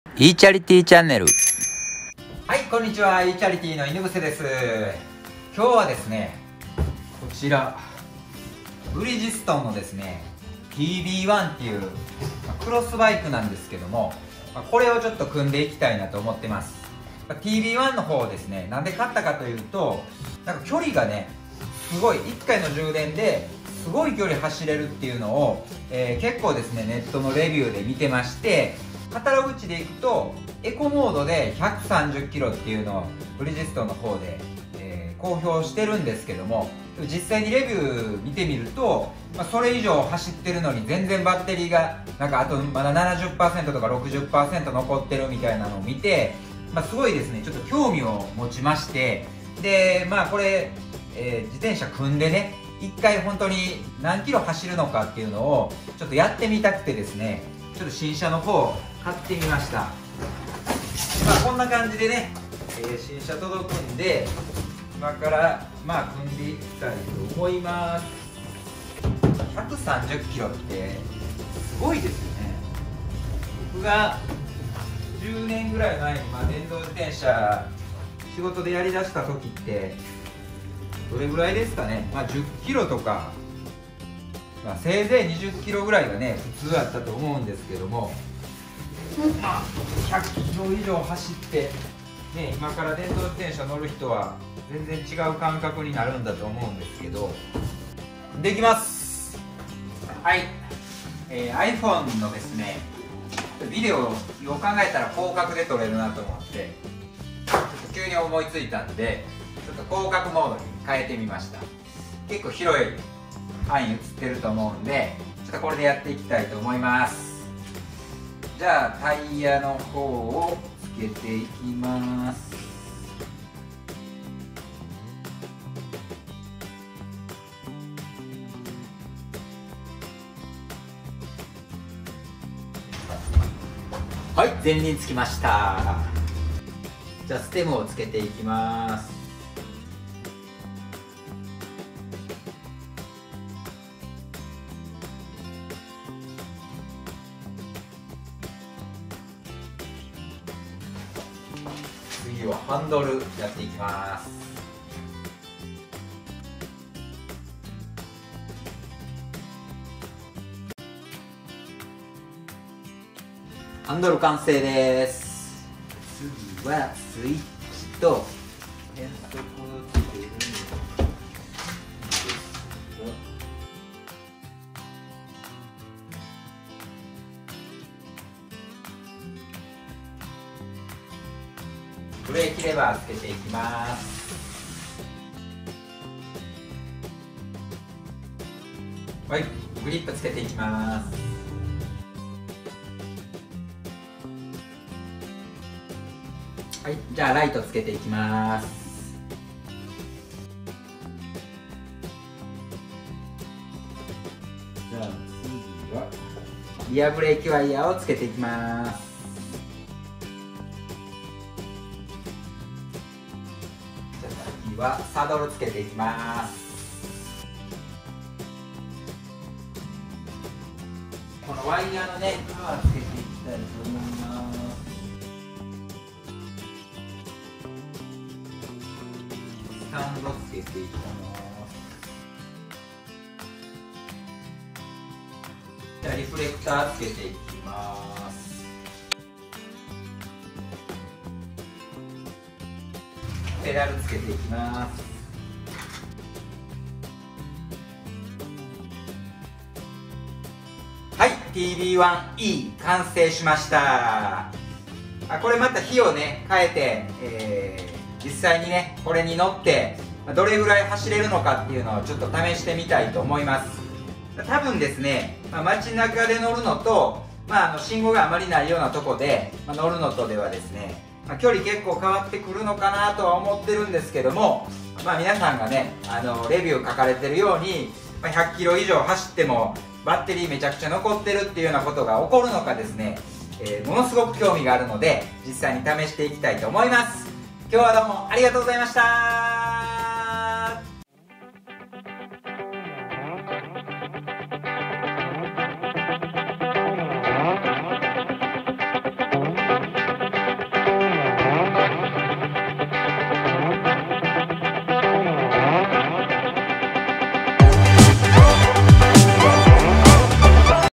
ブリヂストンTB1e組立て編【2020モデルレビュー】
長距離を走行できると評判のブリヂストンTB1e。
はたしてその実力は！？
今回は新車の組立てをやっていきます。